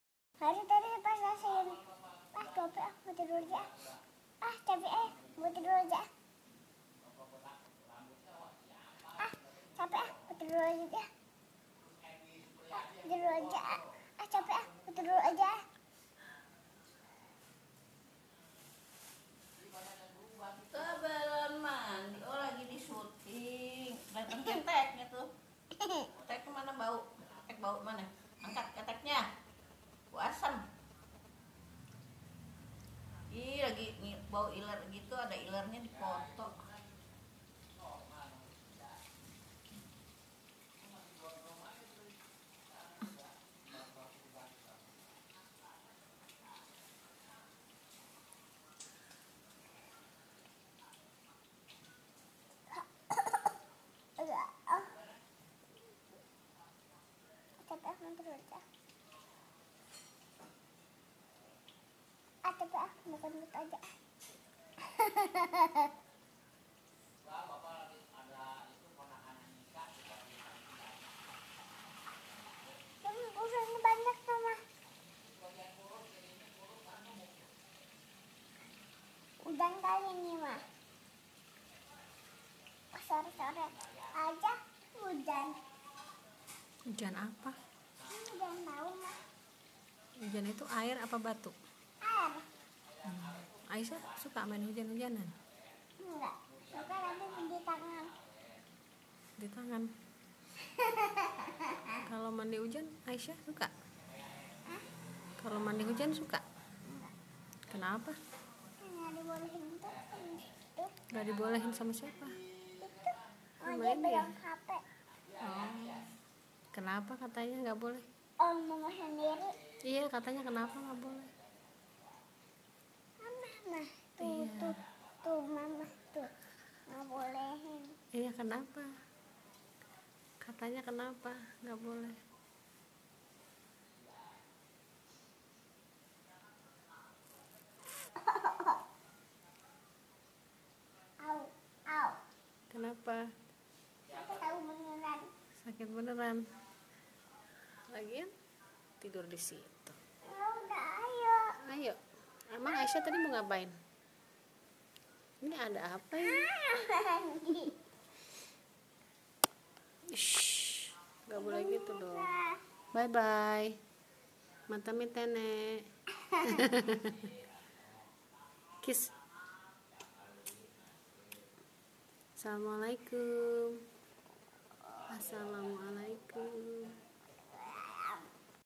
Aisyah ngobrol sama mama
Aisyah ngobrol sama mama sebelum mandi